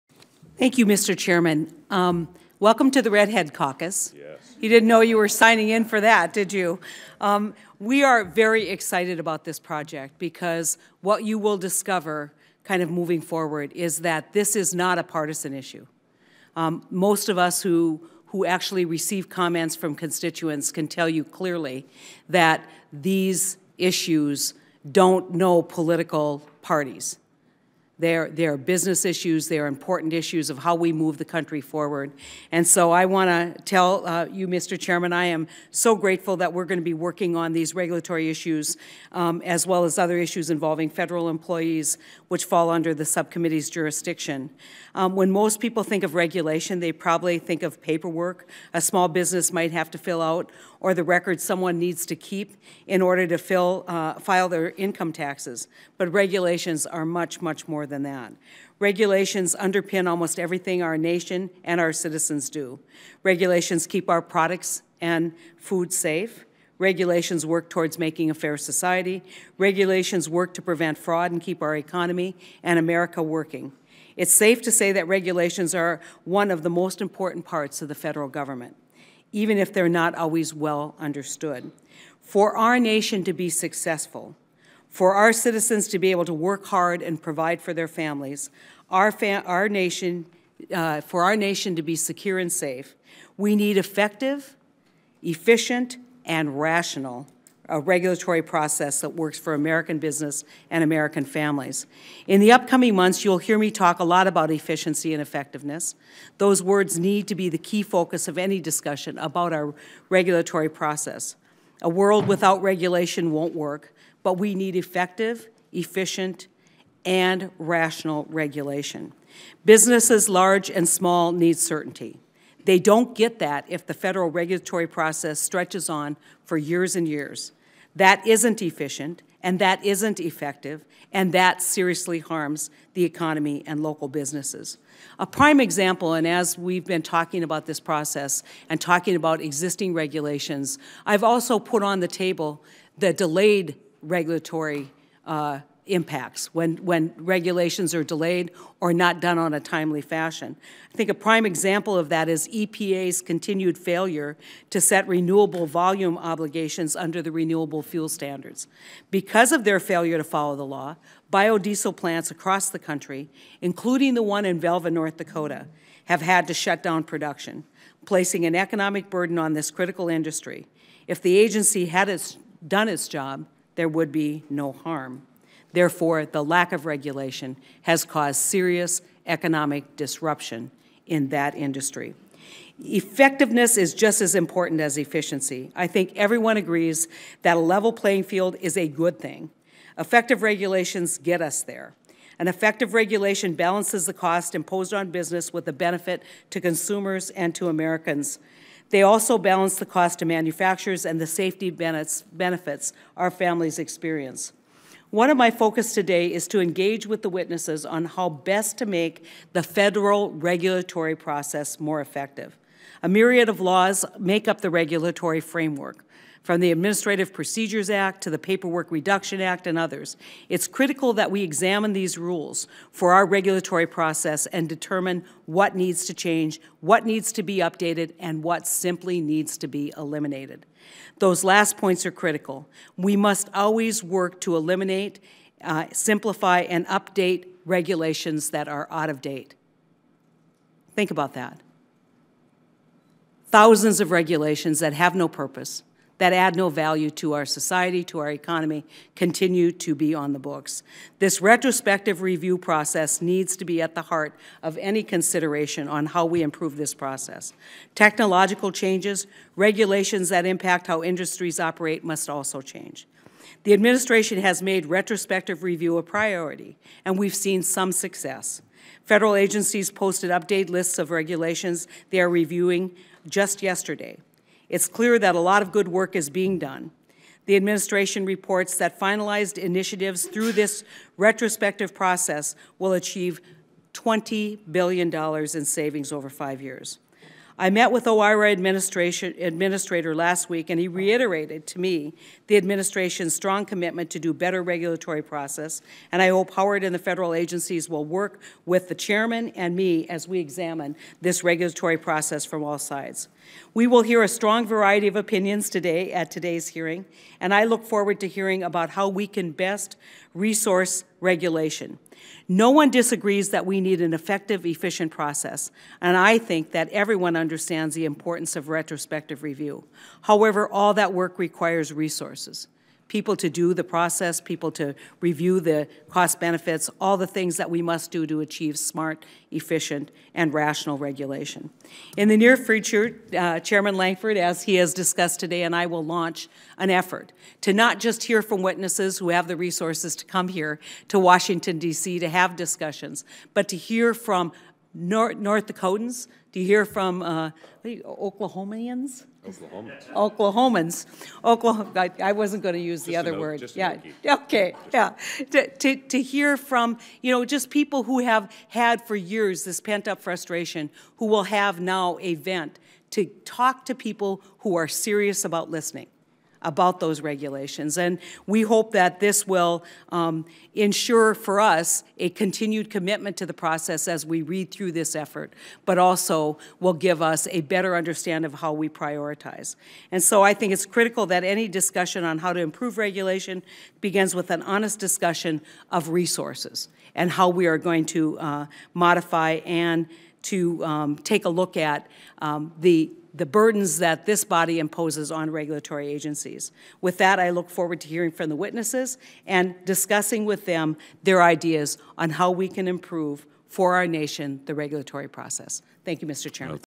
Ranking Member Heitkamp Gives her Opening Statement at Senate Homeland Security Subcommittee Hearing
U.S. Senator Heidi Heitkamp, Ranking Member of the Senate Committee on Homeland Security and Governmental Affairs Subcommittee on Regulatory Affairs and Federal Management, gives an opening statement at the subcommittee's first hearing in the 114th Congress on March 19, 2015.